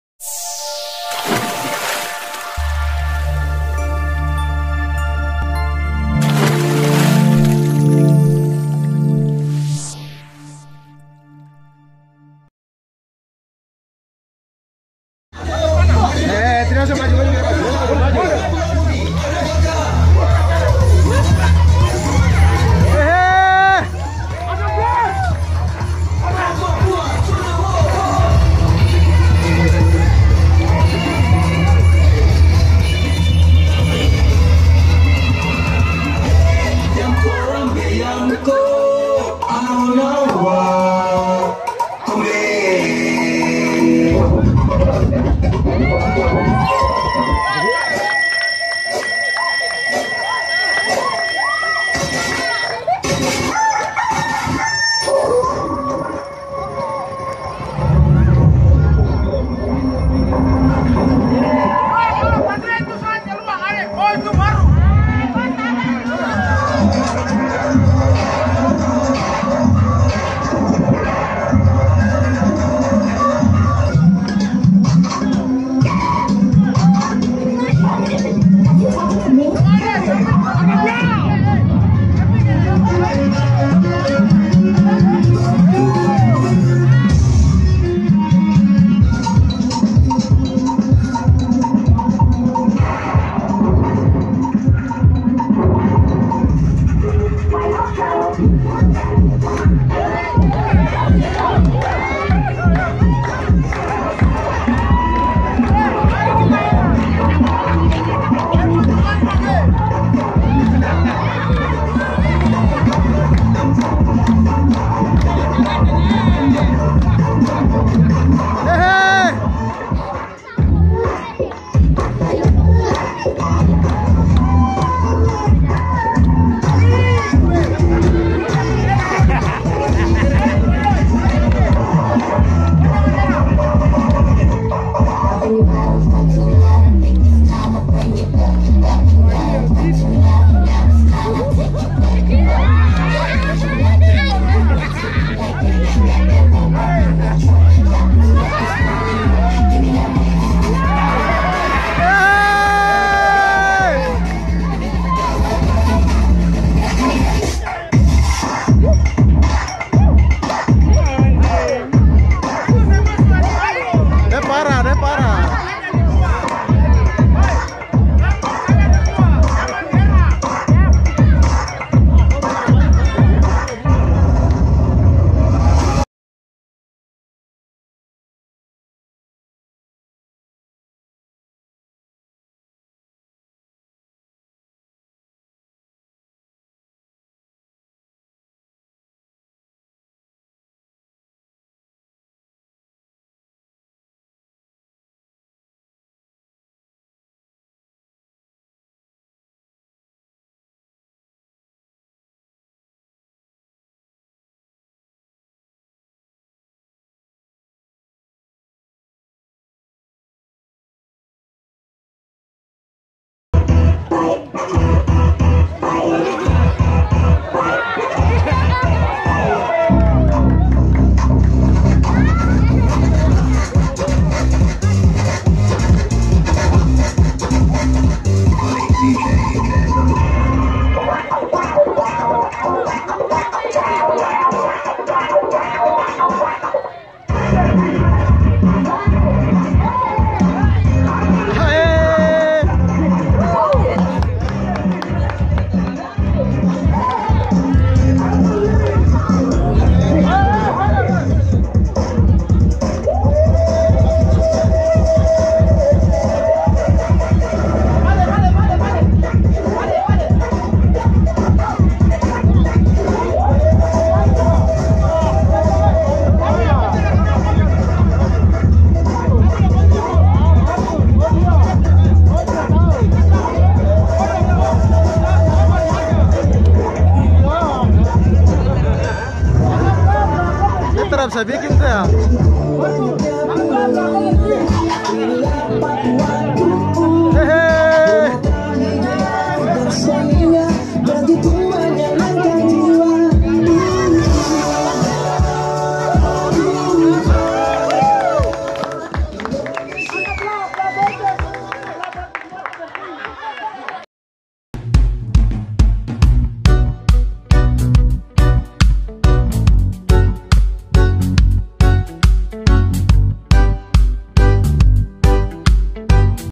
New City Dancer_ Paniai
Turnamen Futsall yang di Gelar di lapangan RSUD Paniai.
New City Dancer Ikut memeriahkan Acara penutupan Piala Bupati tersebut. Yang mana keluar sebagai juara satu adalah ABDI PRAJA. FC. Slamat menyaksikan 🙏🎥